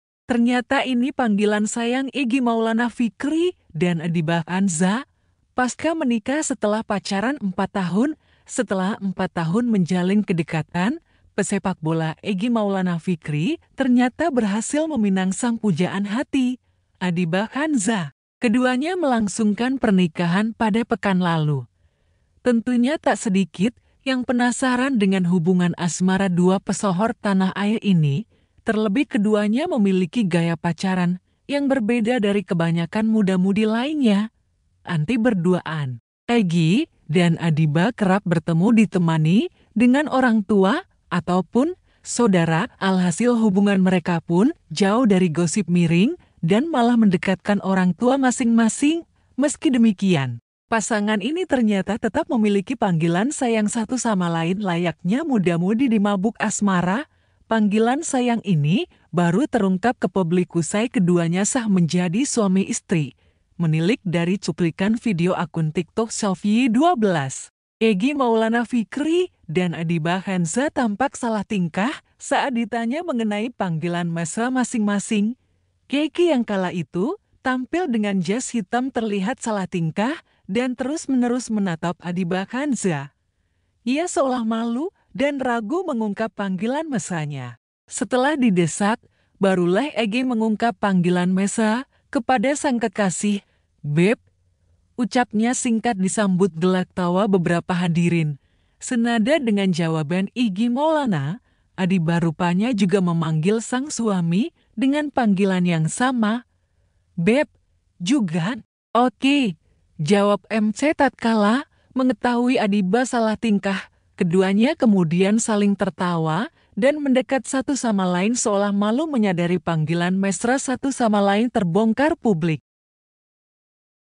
“Ternyata Ini Panggilan Sayang Egy Maulana Vikri dan Adiba Khanza.”
“Ternyata Ini Panggilan Sayang Egy Maulana Vikri dan Adiba Khanza.” Pasca Menikah Setelah Pacaran 4 Tahun.

Setelah empat tahun menjalin kedekatan, pesepak bola Egy Maulana Vikri ternyata berhasil meminang sang pujaan hati, Adiba Khanza. Keduanya melangsungkan pernikahan pada pekan lalu.

Tentunya tak sedikit yang penasaran dengan hubungan asmara dua pesohor tanah air ini. Terlebih keduanya memiliki gaya pacaran yang berbeda dari kebanyakan muda mudi lainnya.